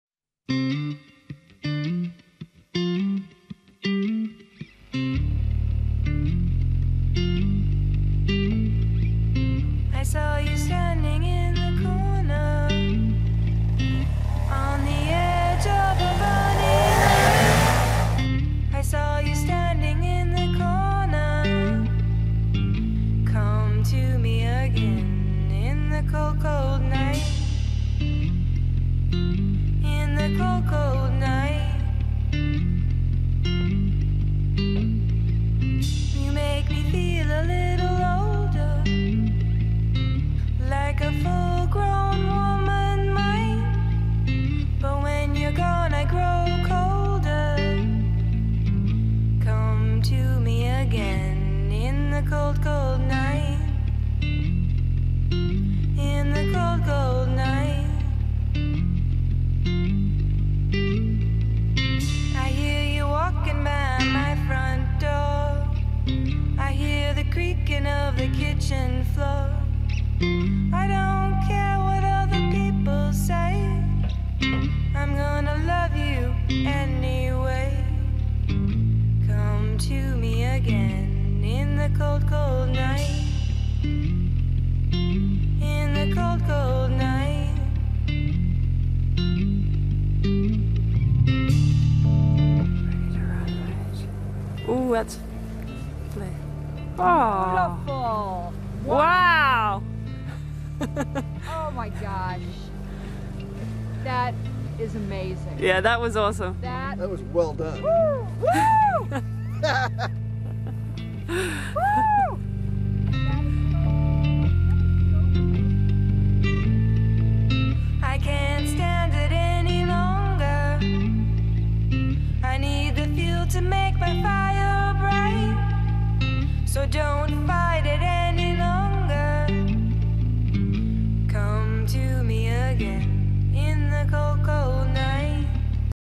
Paramotoring Whitefish Montana in winter
This was my 21st flight and my first failed forward launch. The super cold air seems to do something to the wing and I could not get it to come up straight, it also wanted to come down easily which is why I failed the first launch attempt.

But I managed to get up in the -10 degree celsius air and had an extremely cold flight for about 10 minutes because my feet were going numb and my eyeballs freezing up.

It was beautiful paramotoring over the snowy fields spotting deer and looking out towards Glacier National Park. Unfortunately my Gopro switched off (probably froze) so no footage from the sky.

Thanks to Bob and Wendy for being my ground crew and motivators (and for letting us stay in their amaaaaaazing cottage at the lodge in Whitefish

I am flying a Blackhawk BH125 Paramotor and the Velocity Elektra 28m Paraglider.